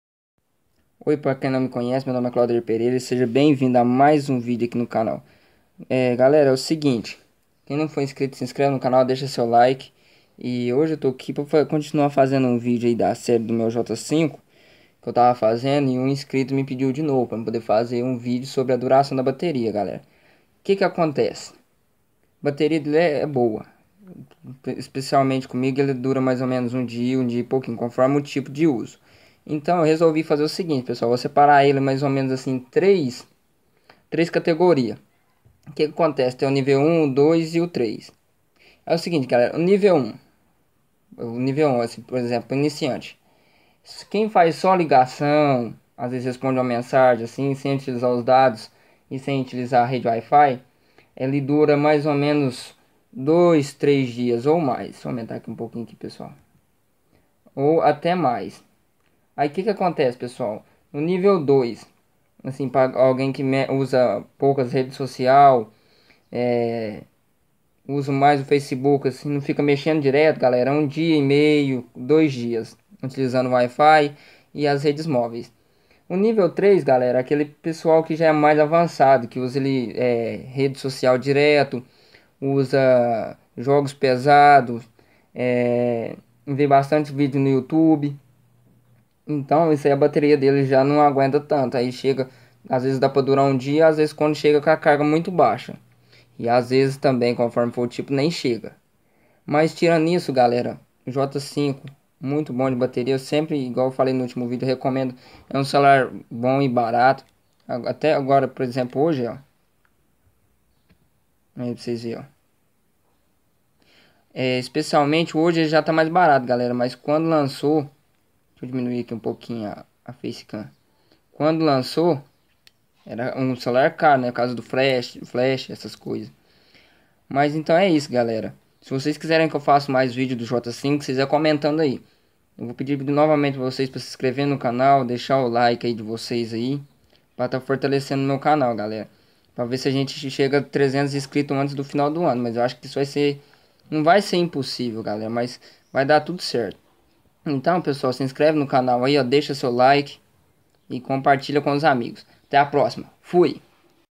Tudo sobre a bateria do meu galaxy J5 (respondeno duvida de um inscrito)
Tudo sobre a bateria do meu galaxy J5 (respondendo duvida de um inscrito) se vocês quiserem mais videos ,so vocês me pedirem...